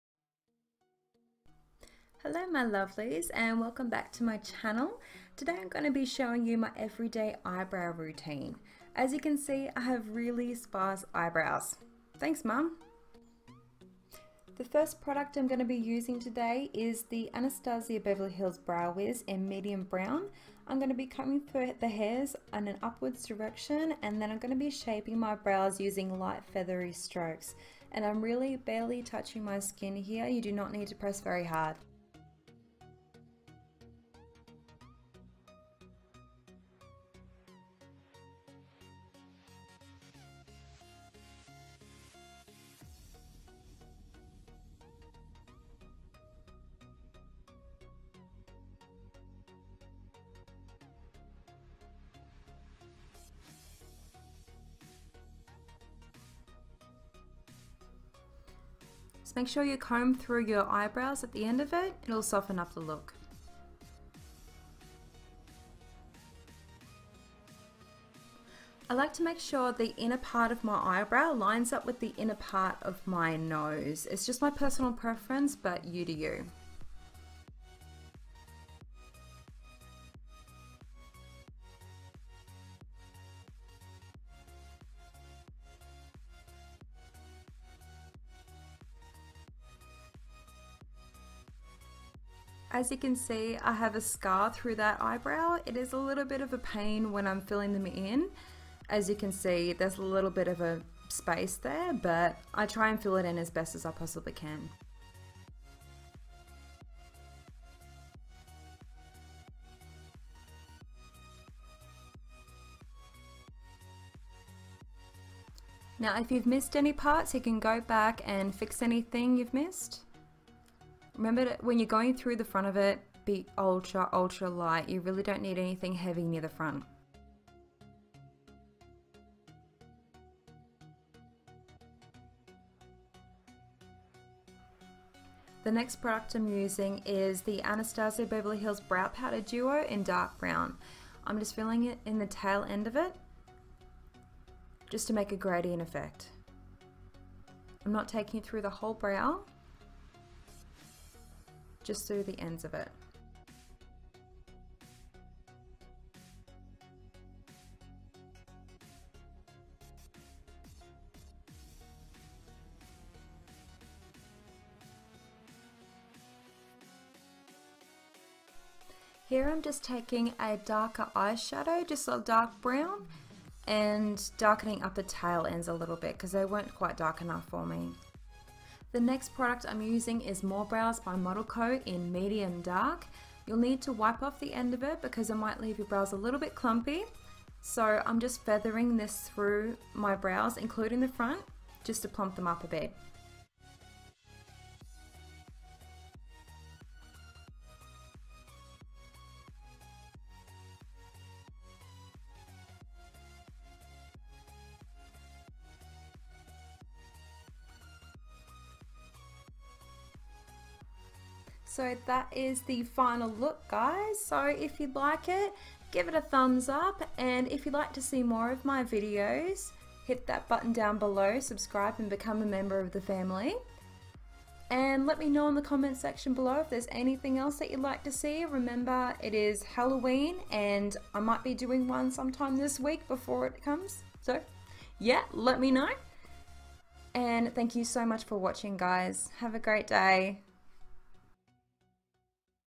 My Eyebrow Routine
Hey guys!

I wanted to try a voiceover today because I got a new microphone. I kinda like it because I don't have to try and edit out all the "ums" haha.
I'll be trying out a Halloween look tomorrow so keep an eye out for it in the next few days!